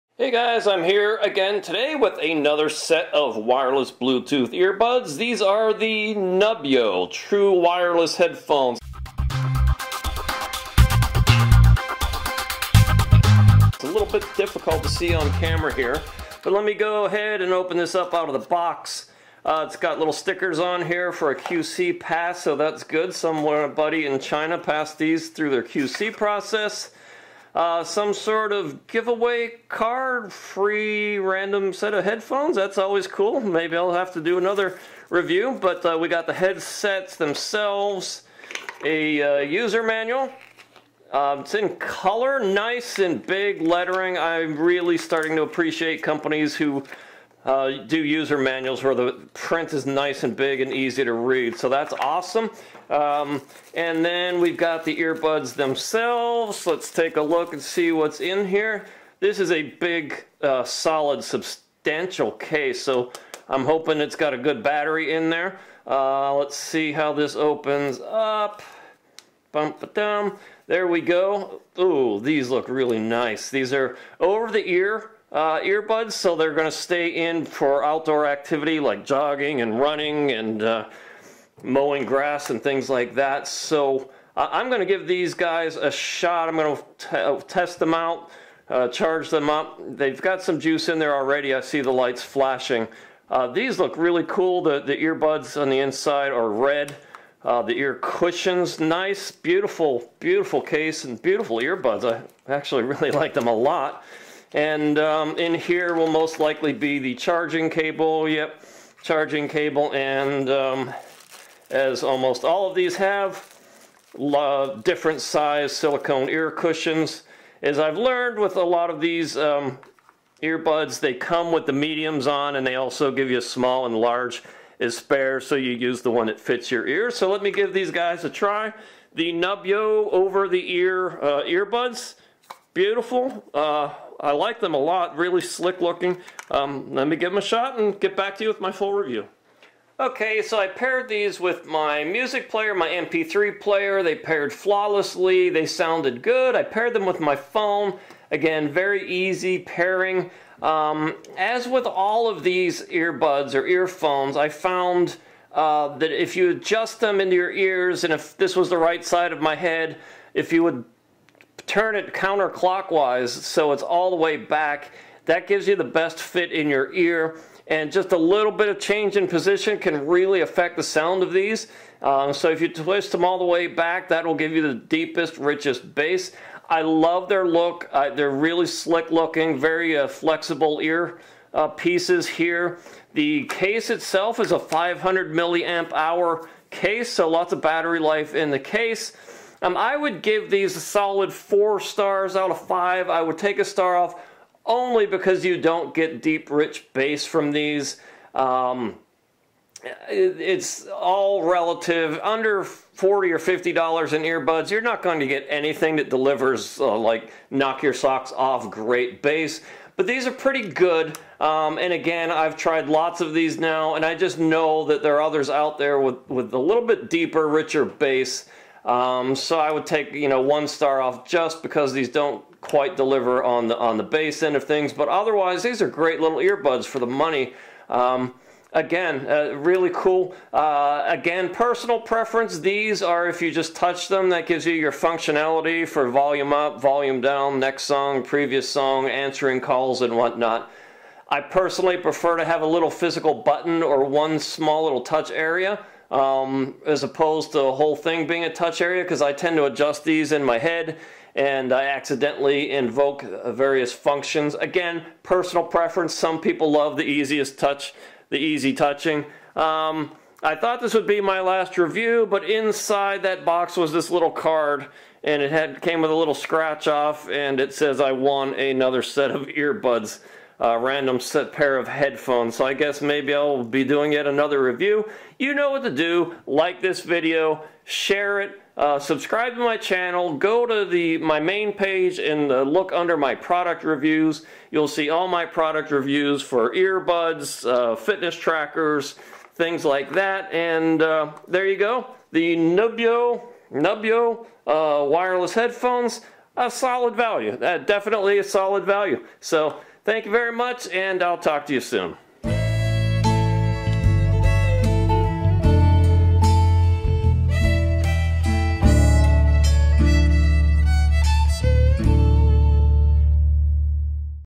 NUBBYO Wireless Headphones review
Check them out on Amazon

Follow-up June 4, 2020: I have since put these through my unique "lawnmower test" when I wear the headphones while mowing my grass using a push mower.  (For some reason lawnmowers seem to create a lot of interference and make BLE connection more difficult.)  These did not pass the test.  With my mp3 player in my pocket these repeatedly kept losing the connection.  Under all other conditions so far they work great, though.